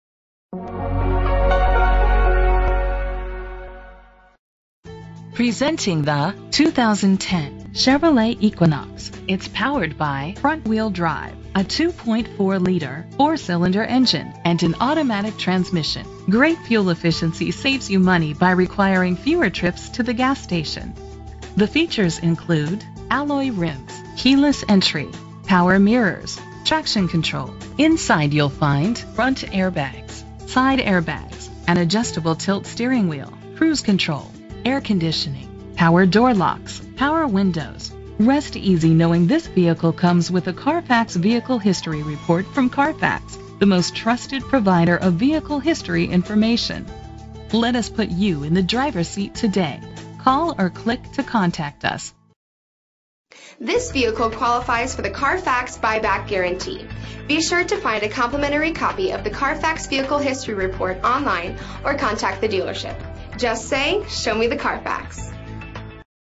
2010 Chevrolet Equinox F7123 - Tinley Park IL
Year: 2010
Make: Chevrolet
Model: Equinox
Trim: LT
Engine: 2.4 liter 4 Cylindercylinder FWD
Transmission: Automatic
Color: White
Mileage: 51912
Address:
17514 Oak Park Avenue
Tinley Park, IL 60477
VIN:2CNALDEW4A6207902